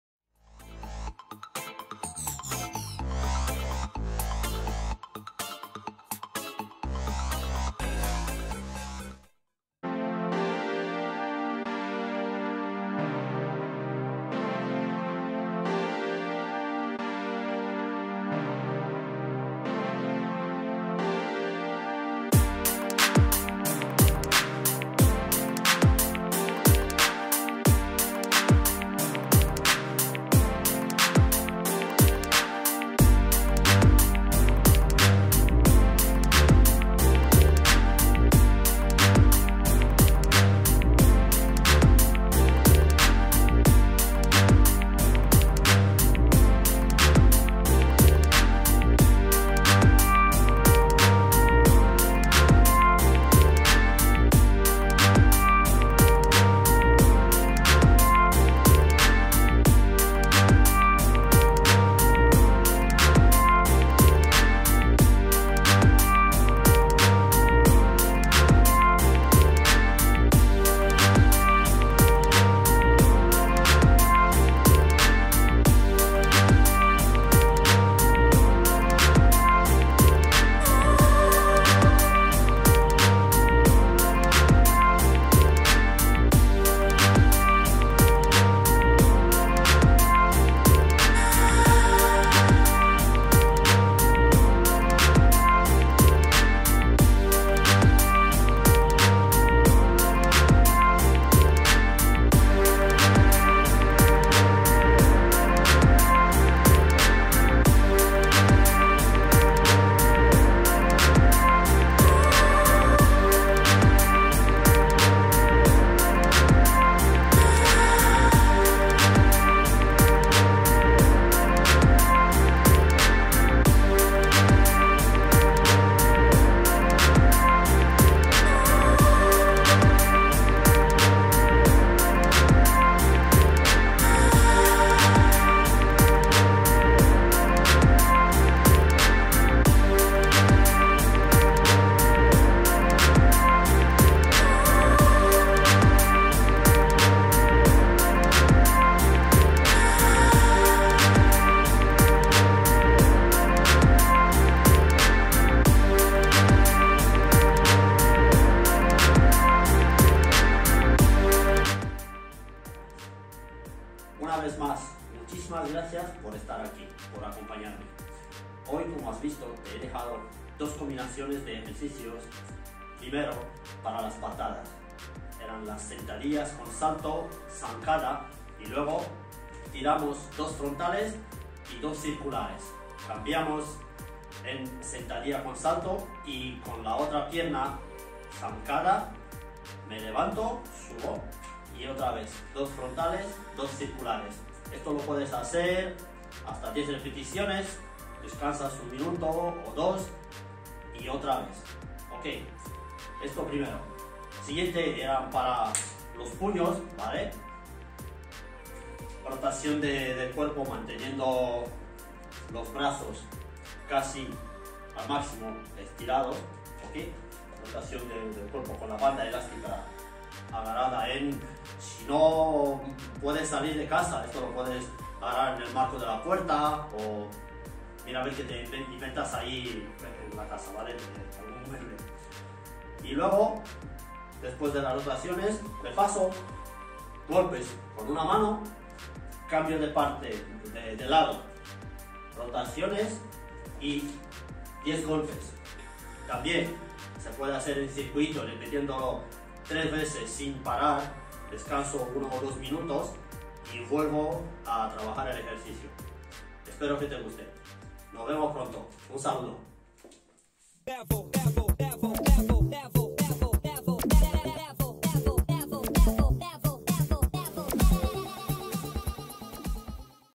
💥EXPLOSIÓN💥 ejercicios para golpear ¡MÁS FUERTE!
¡Hola amigo!
Hoy te dejo dos ejercicios para aumentar la potencia de tus golpes. Son dos mini circuitos o super series, en que enlazamos golpes con movimientos de fuerza explosiva.
Espero que te ayuden
¡Un saludo!